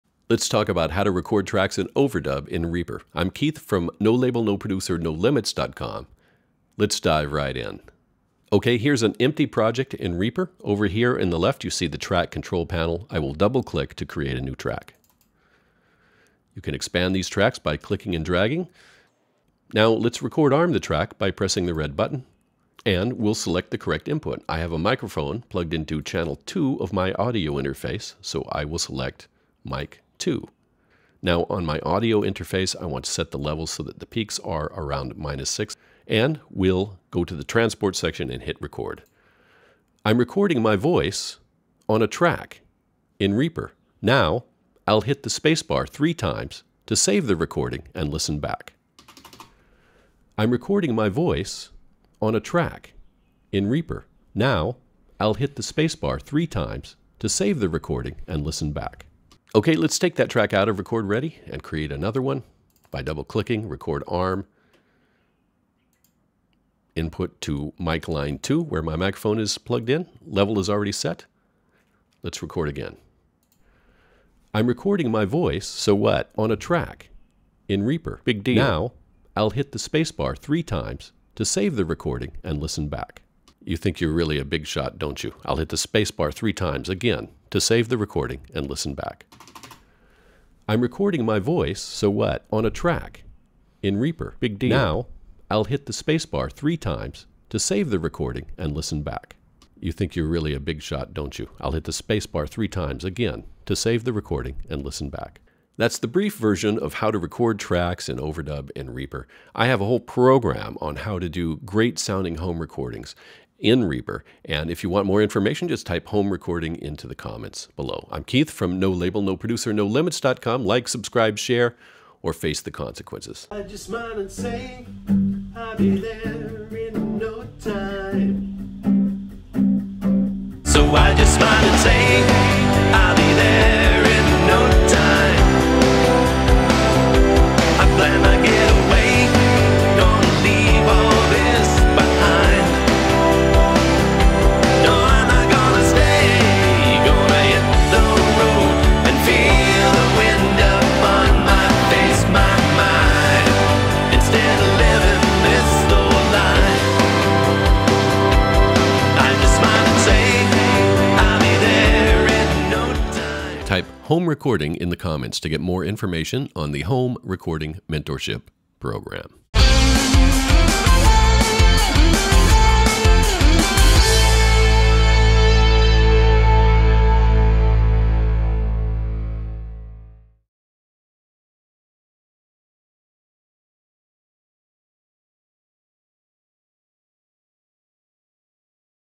Recording & Overdubbing In Reaper
How to record & overdub in Reaper in 3 minutes.

Gear List For Home Studio
Audient EVO Interface

Slate VSX Headphones & Room Modeling

Presonus Eris 3.5 Studio Monitors

Mics

Shure SM57

Shure SM7b

Aston Spirit